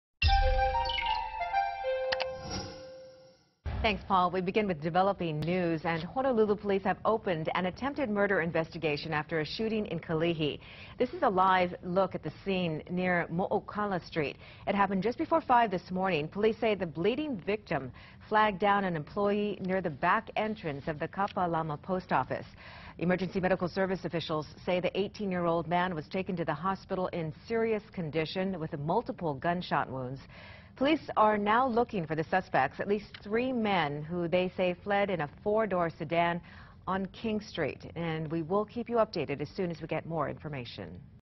Honolulu police investigate Kalihi shooting
An attempted murder case has been opened Saturday morning after a man who shot near the Kapalama Post Office in Kalihi. For news you need to know,